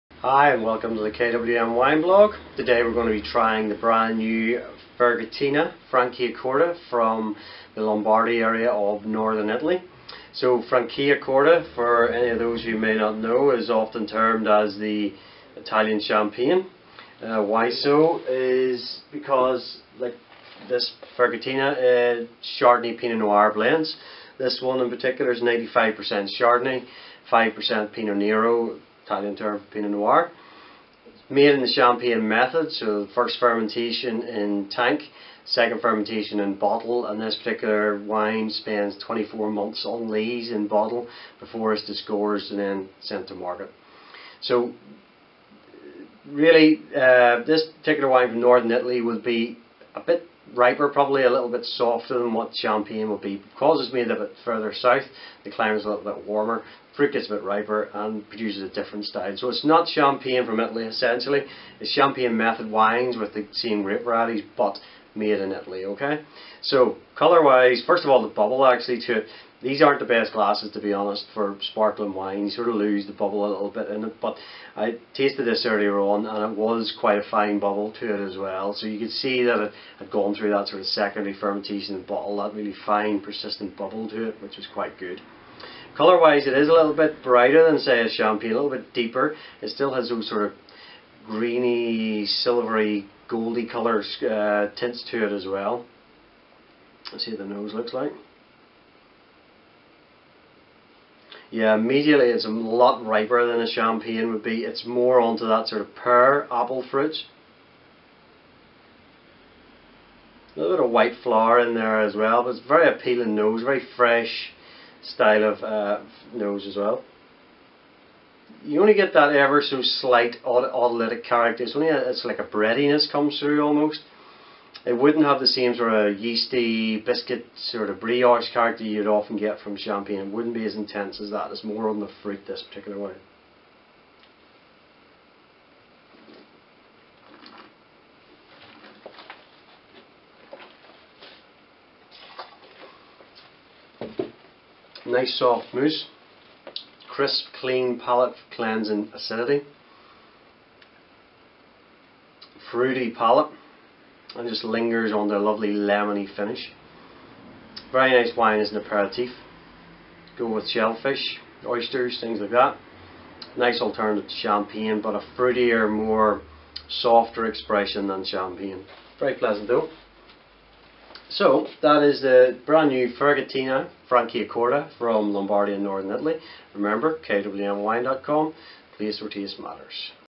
Ferghettina Franciacorta Brut NV
Video tasting of Ferghettina Franciacorta Brut NV by Andrew Imrie at KWM Wines & Spirits Online Wine & Spirits Merchants shipping throughout the UK, Ireland, Europe and the World.